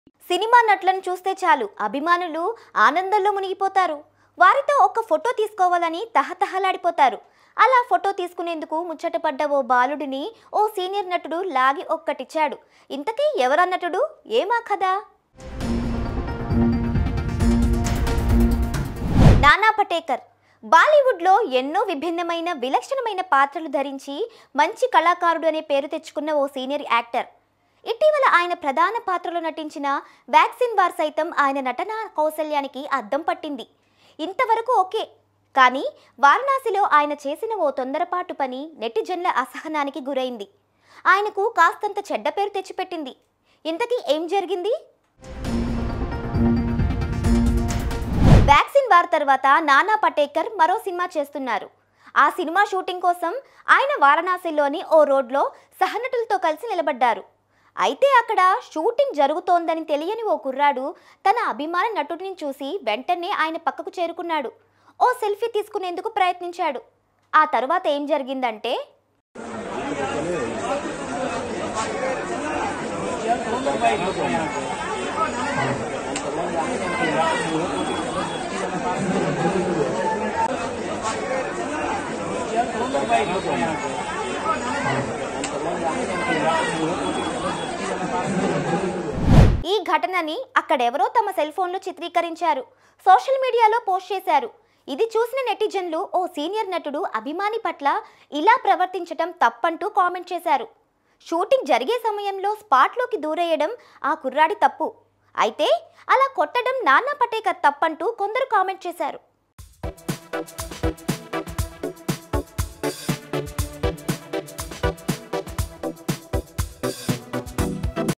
ఫ్యాన్స్ ని కొట్టిన సీనియర్ నటుడు Senior actor Nana Patekar Hitting A Fan On Road@SumanTVNalgonda
Watch ►► ఫ్యాన్స్ ని కొట్టిన సీనియర్ నటుడు
Senior actor Nana Patekar Hitting A Fan